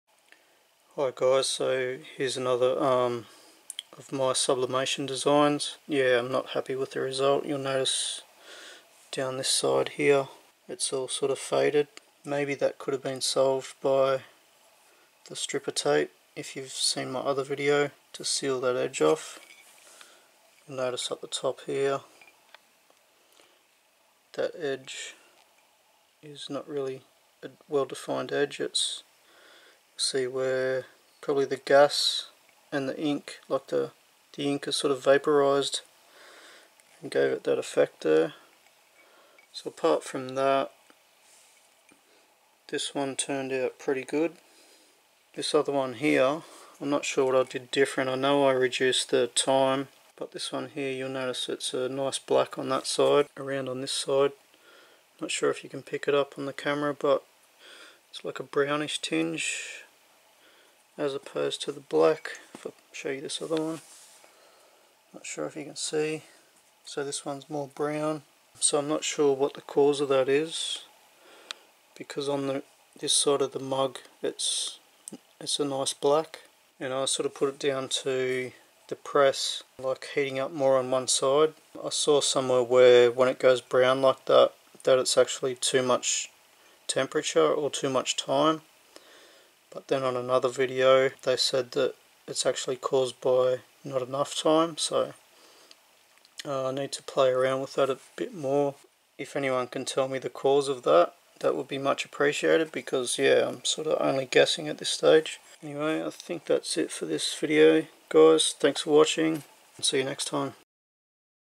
Mug sublimation problem. Black ink turning brown. Please help!
A video demonstrating an issue with black ink appearing brown when trying to sublimate large areas of black onto a mug.

I would really appreciate if any experienced sublimators could give me advice on how to rectify this issue.